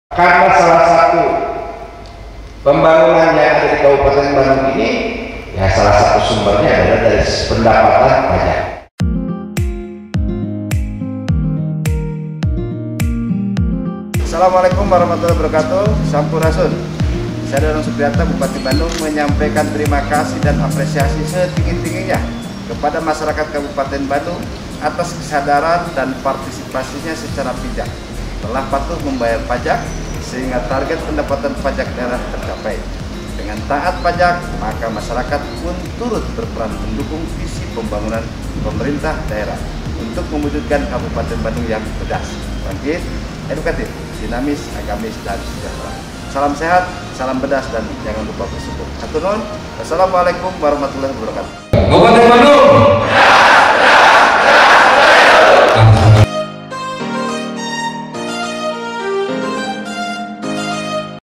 Bupati Bandung Minta Para Kades Lancarkan Program PTSL
Sebagai informasi, program PTSL adalah proses pendaftaran tanah untuk pertama kali, yang dilakukan secara serentak dan meliputi semua obyek pendaftaran tanah yang belum didaftarkan di dalam suatu wilayah desa atau kelurahan atau nama lainnya yang setingkat dengan itu. Melalui program ini, pemerintah memberikan jaminan kepastian hukum atau hak atas tanah yang dimiliki masyarakat.

Selain itu, bupati mengimbau kepada para kader pendapatan untuk tidak menerima uang. “Saya menyarankan para kader ini memberikan dan memberitahukan kepada para wajib pajak dengan membawa SPPTnya. Selanjutnya, para wajib pajak ini dapat membayar langsung ke bank atau aplikasi uang digital,” ujar bupati.

Bupati pun menyebutkan bahwa hanya para kader pendapatan saja yang boleh menyampaikan SPPT.

“Yang jelas tolong dibantu kepada teman-teman di lapangan, karena salah satu pembangunan yang ada di Kabupaten Bandung ini salah satu sumbernya dari pendapatan pajak,” tambahnya.

Selama melaksanakan tugas dengan sebaik-baiknya, bupati yakin melalui kolaborasi dan kompak, program yang akan dilakukan dapat berhasil dan sukses bagi kemajuan Kabupaten Bandung.

Fotografer: @revoliady
Infografis: @teguhputraf
Videografer: @nas.diee
Video editor: @takbirmalik